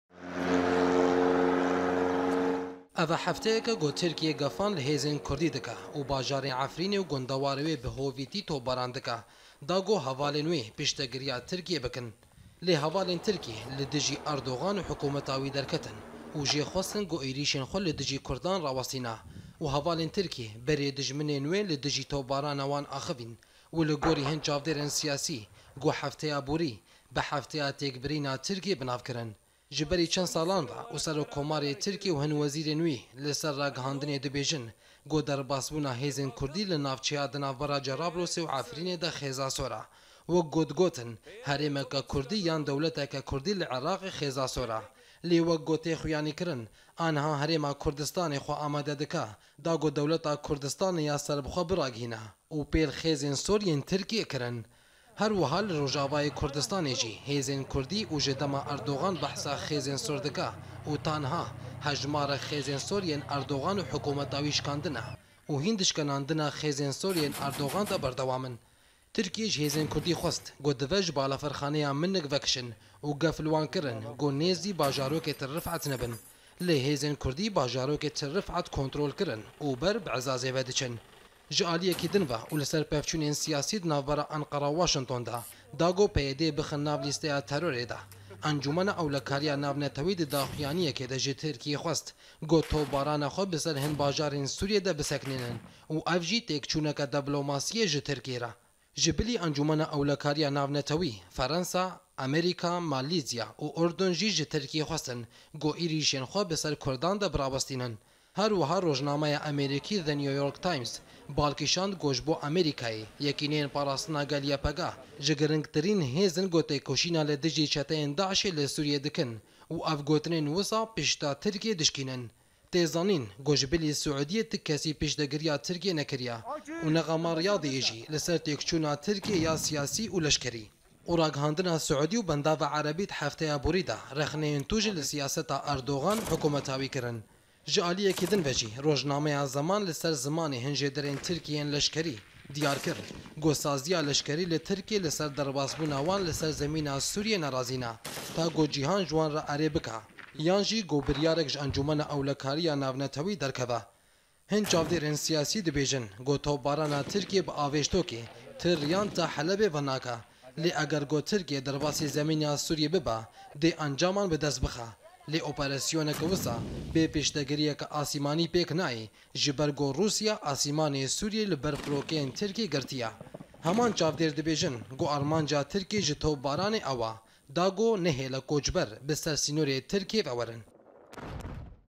Kurdistan Kurdên başûrî û rojava xêzên sor yên Tirkiy şikandin
Di hefteya bûrî de Tirkiyê rastî şikestineke siyasî ,leşkerî û deblomasî hat ,,,dema hevalên Tirkiyê berî dijminên wê nerazîbûna xwe li ser gefên wê bo rojavayê Kurdistanê û bi taybetî li ser navçeyên ku ketine bin destên hêzên Sûriya demoqratîk de anîne ziman û ji Tirkiyê xwestin ku êrîşên xwe rawestînin